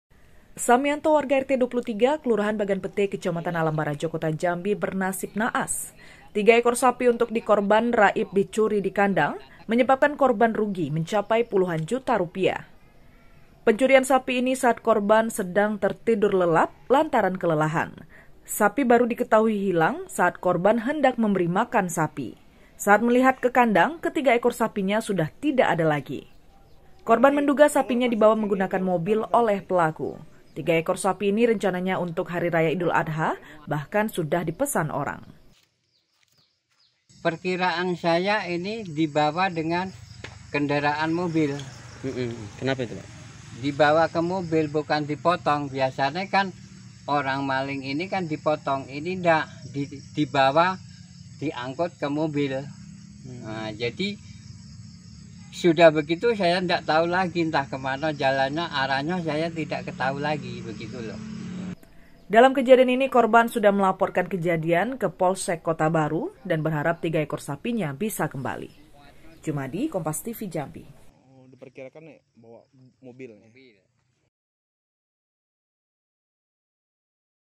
Maling Ternak Sapi
JAMBI, KOMPAS.TV - Sebanyak tiga ekor sapi milik warga seberat tiga ratus kilogram diambil pencuri saat korban sedang lelap tidur, di RT. 23 Kelurahan Bagan Pete Kecamatan Alam Barajo Kota Jambi. Akibat kejadian ini korban mengalami kerugian mencapai puluhan juta rupiah.